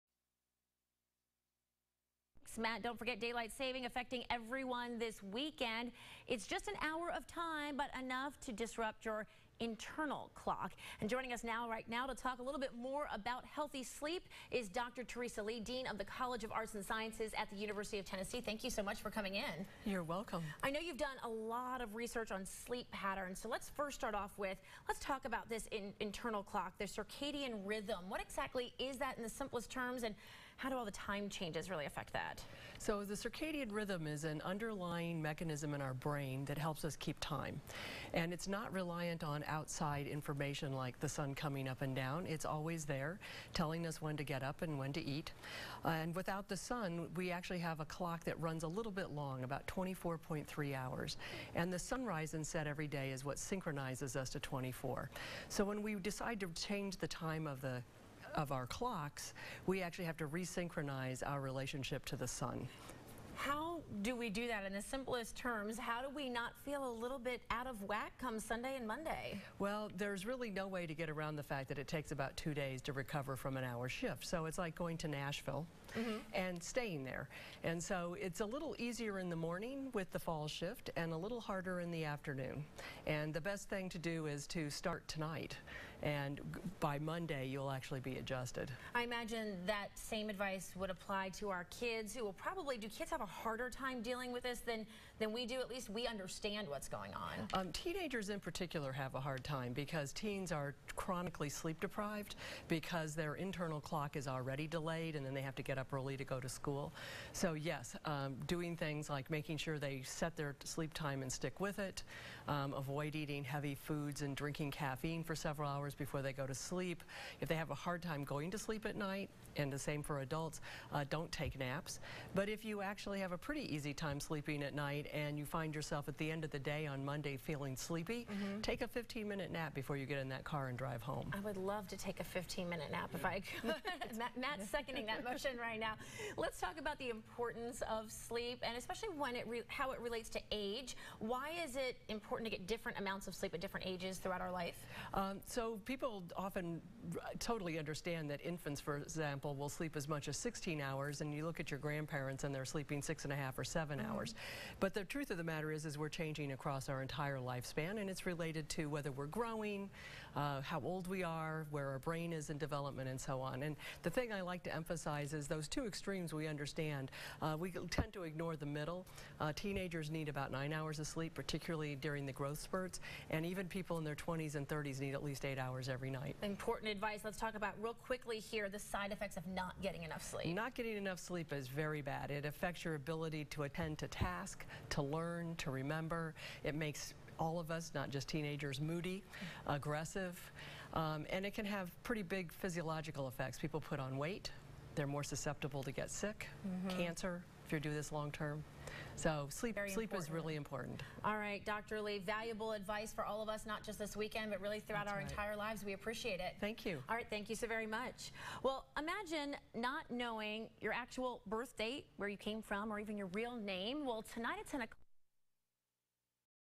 Dean Theresa Lee Interview on WATE
WATE: Lee discusses sleep effects of daylight savings time change